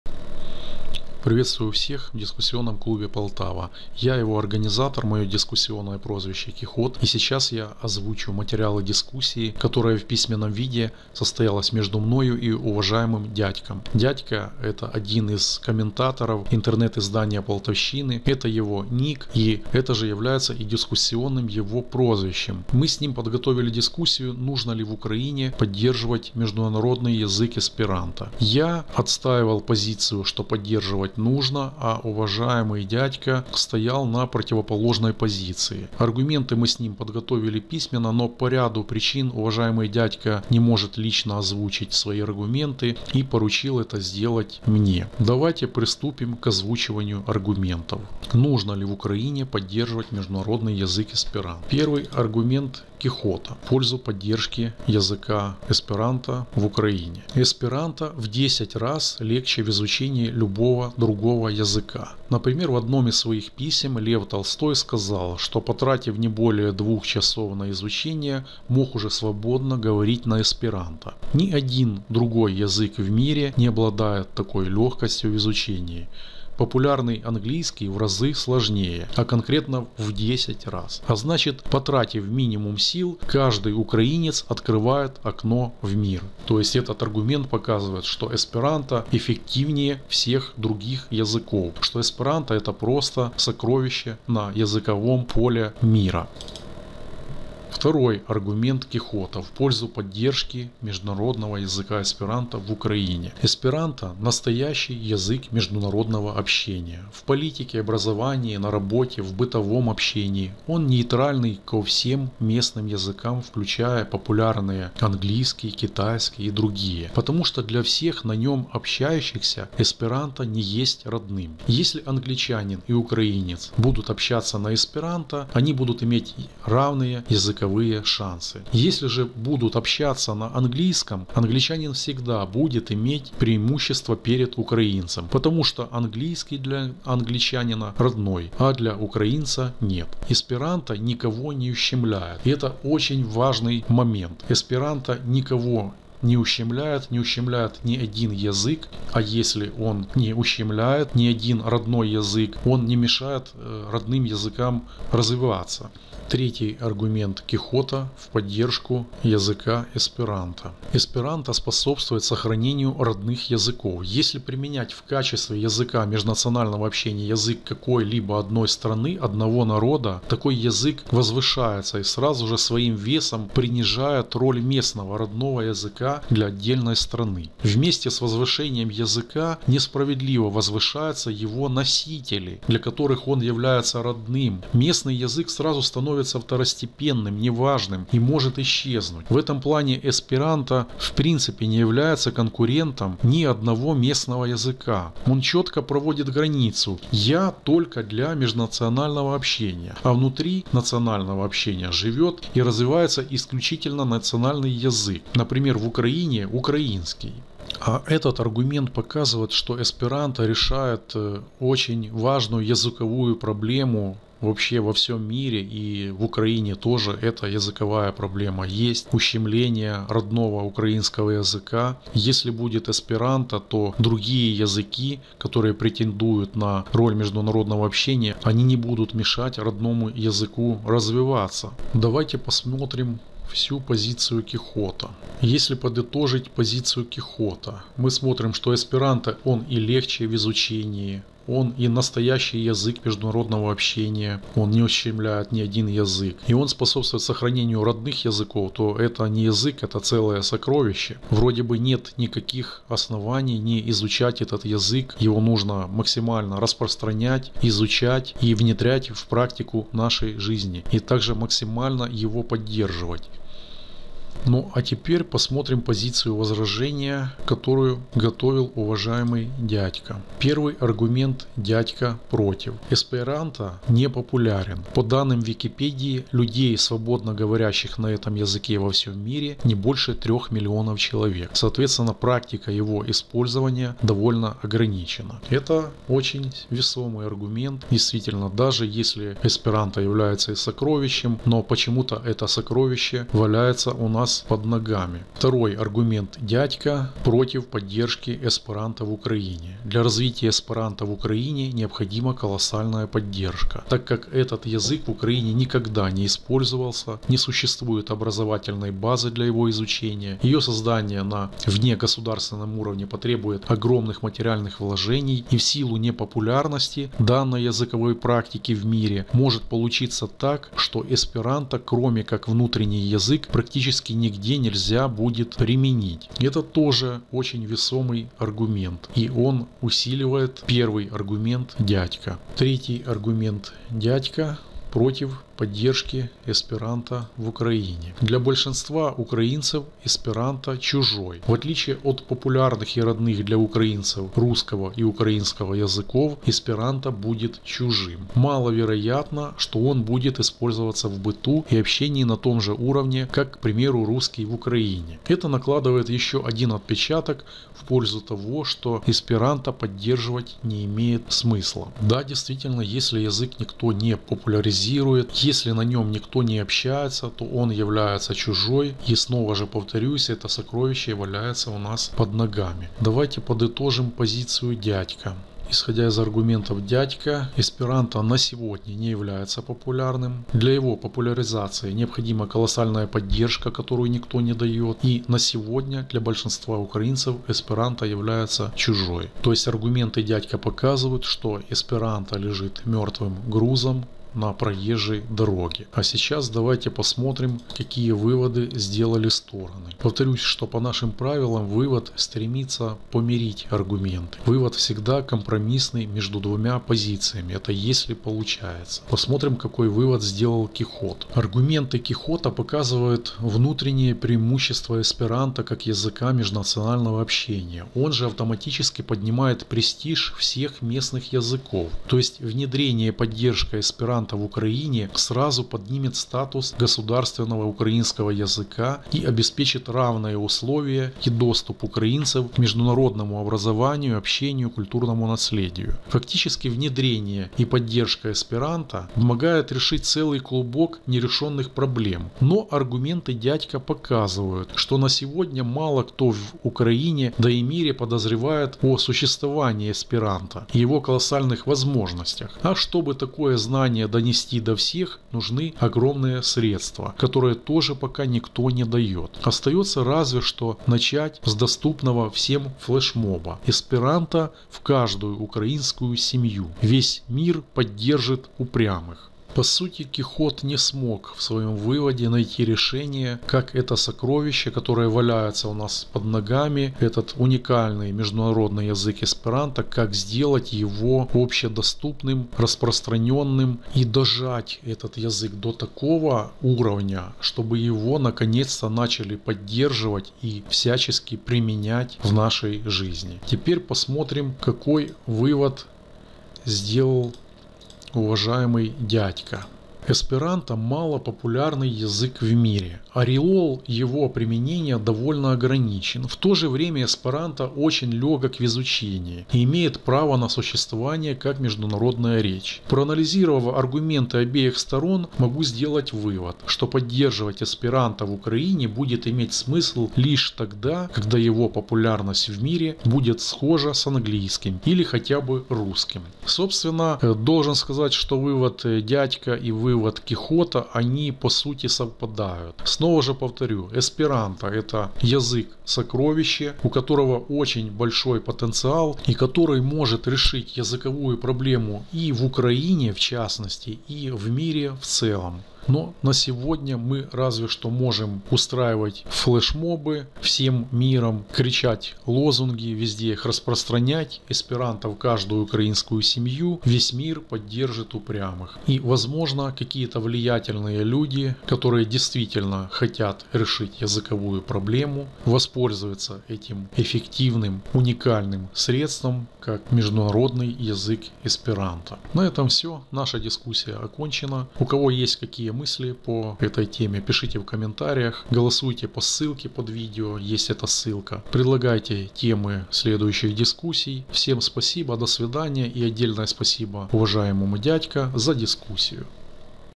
170 ДИСКУССИЯ Нужно ли поддерживать в Украине язык эсперанто 02.05.20
Кихот и Дядько (дискуссионные прозвища) рассмотрели вопрос, нужно ли поддерживать в Украине международный язык эсперанто.
Смотрите нашу дискуссию, размышляйте и делайте свой рассудительный вывод.
Своё мнение можно написать в комментарии.
Проголосовать.